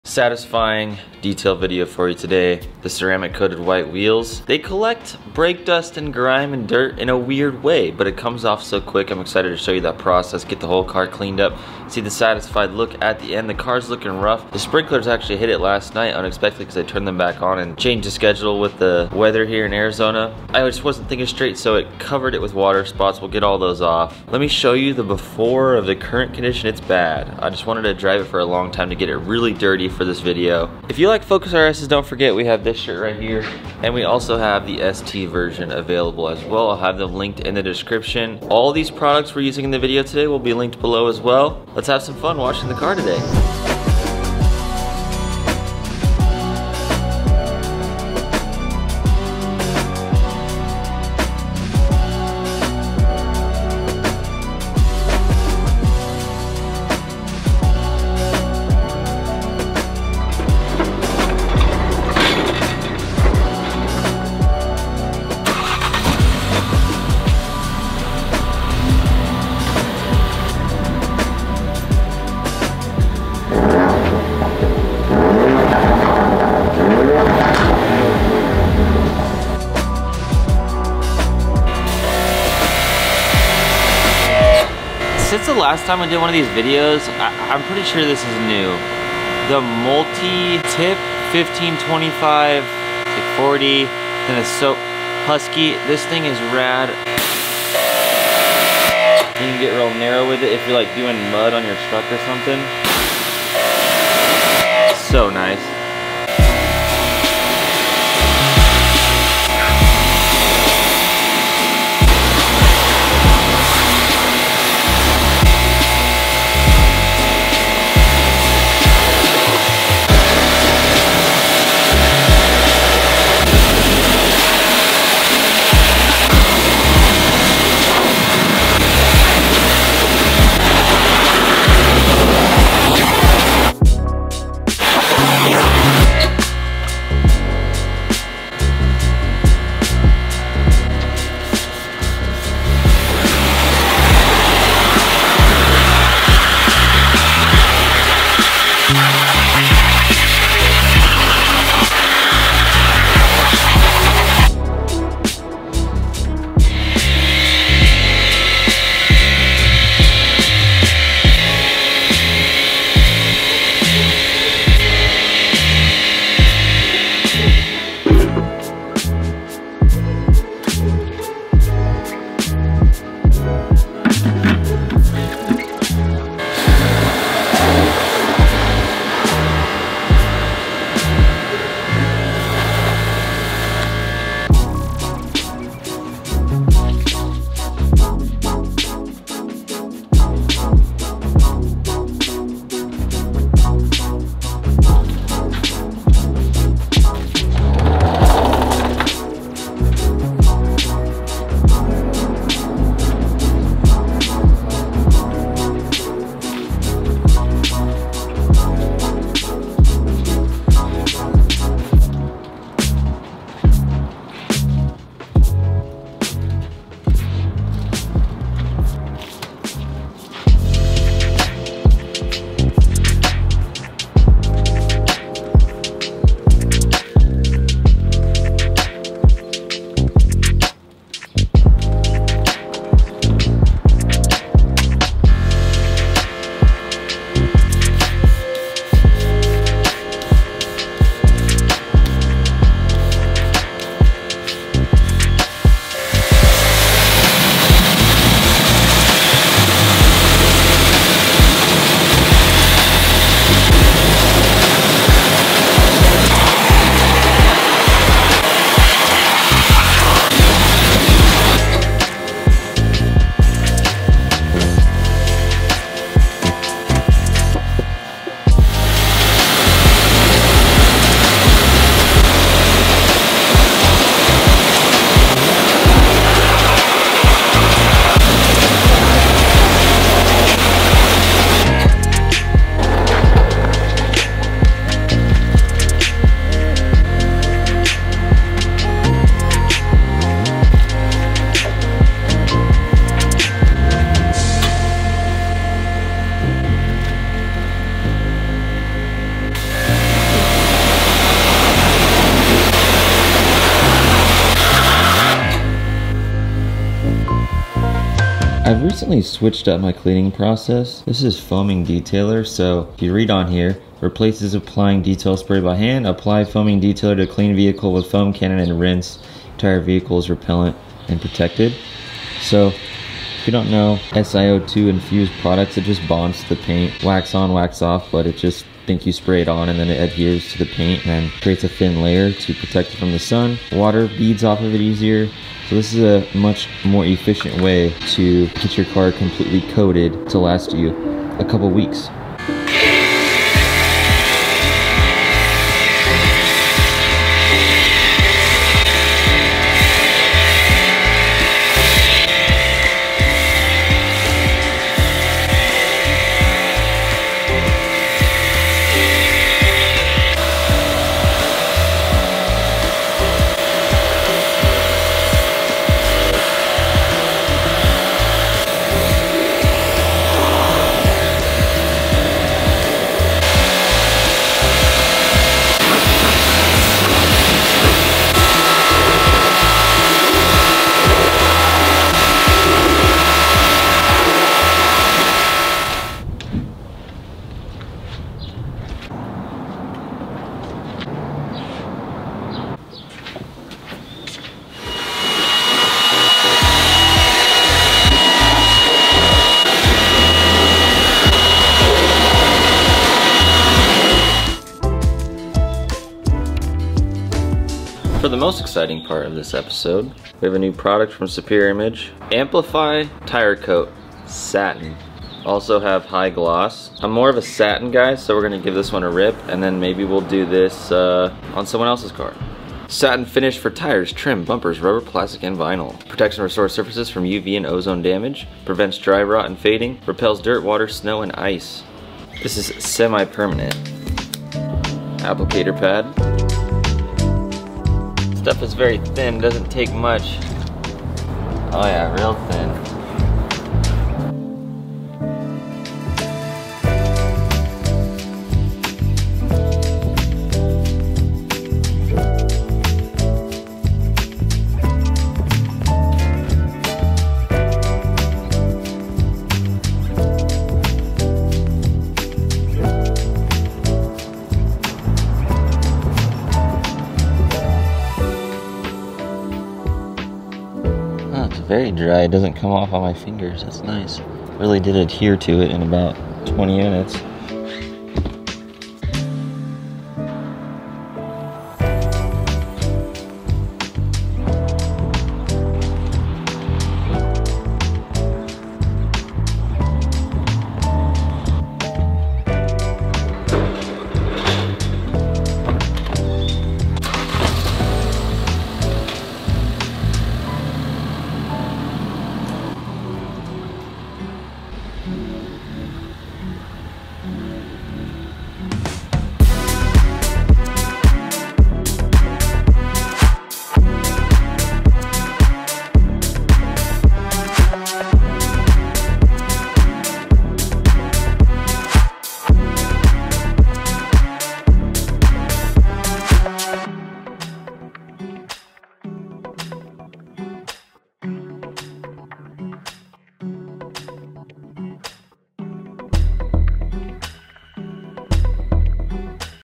Satisfying Ford Focus RS Detail
Chapters:
0:00 Focus RS Detail
0:43 Before
1:01 Pulling Car In
1:24 Nozzle
1:46 Rinse
2:18 This didn't work..
2:34 Maxi-Foam
3:55 Wheel X (This Worked!)
4:43 Wheel Brush
5:08 Foaming Detailer
6:00 Rinse
6:24 Drying
6:48 Tire Coat
7:57 Vanish
8:11 Final Look